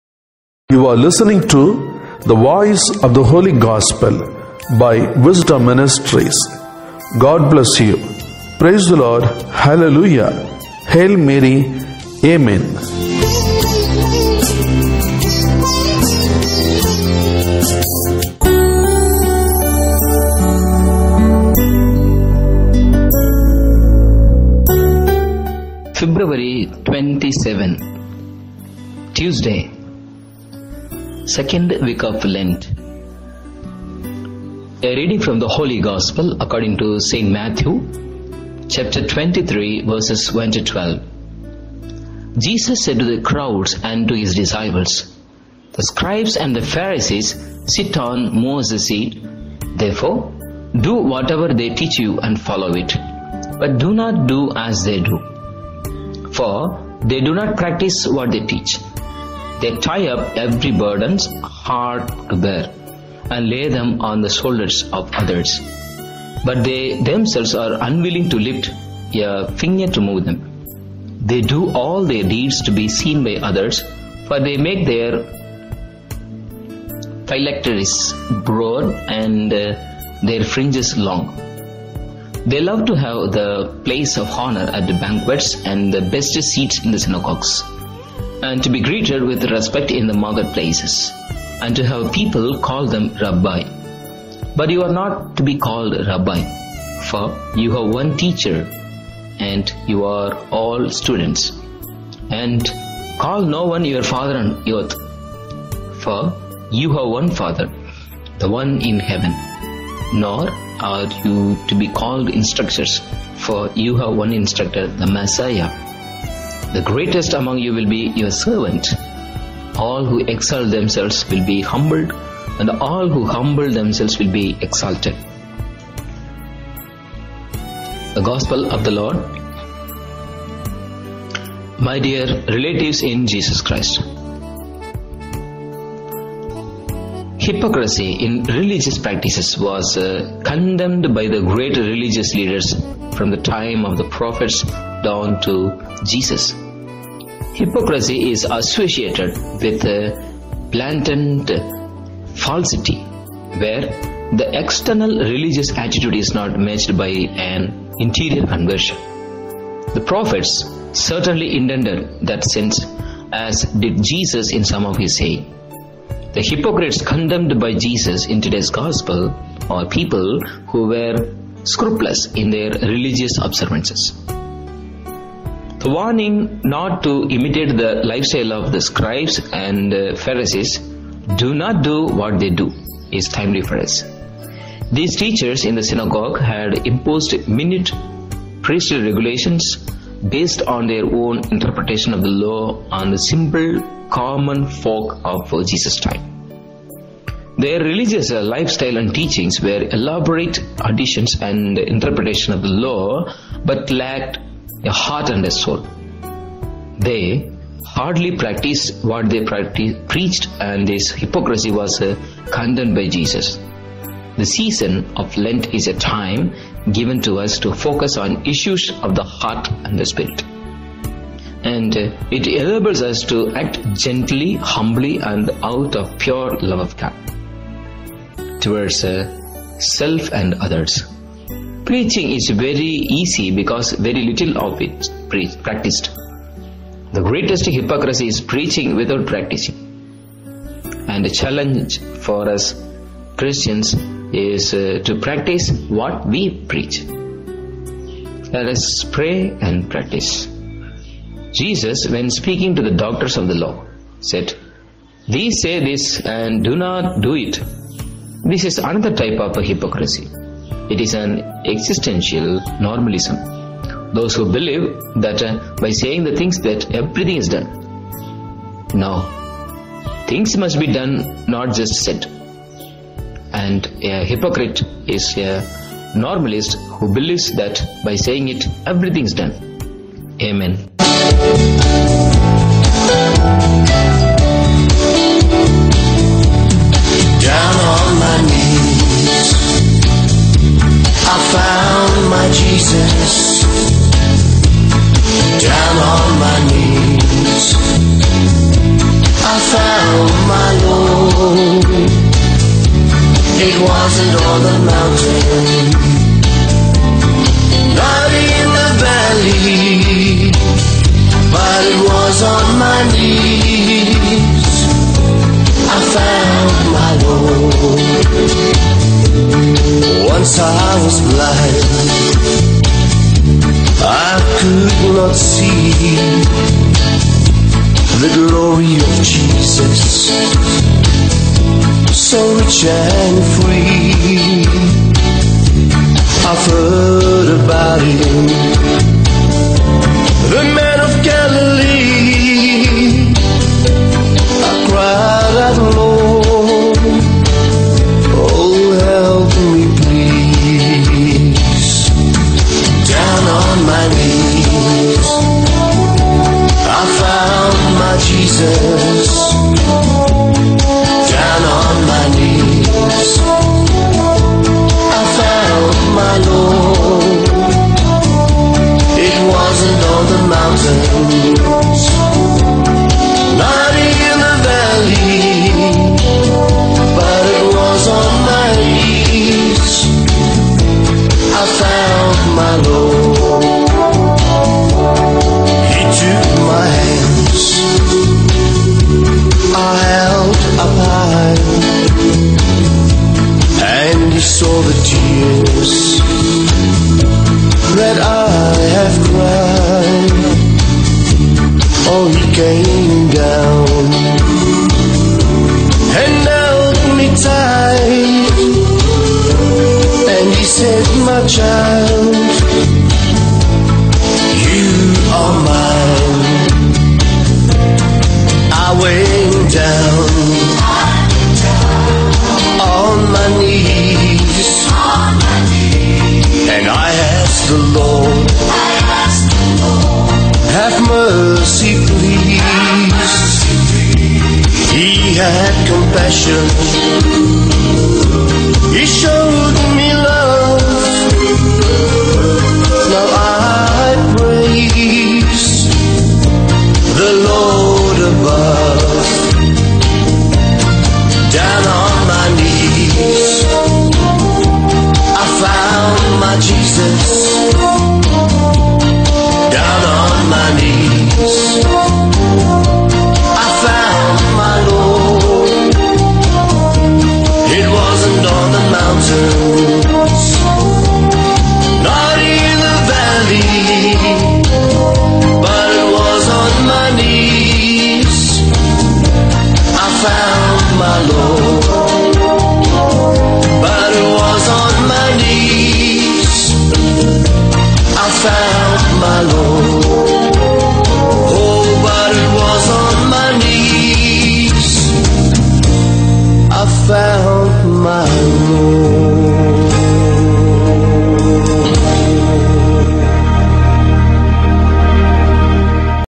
27 02 2018 Gospel of St. Mathew 23:1-12 In English
Daily Gospel for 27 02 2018 In English according to St Mathew 23:1-12 Presented By Wisdom Ministries.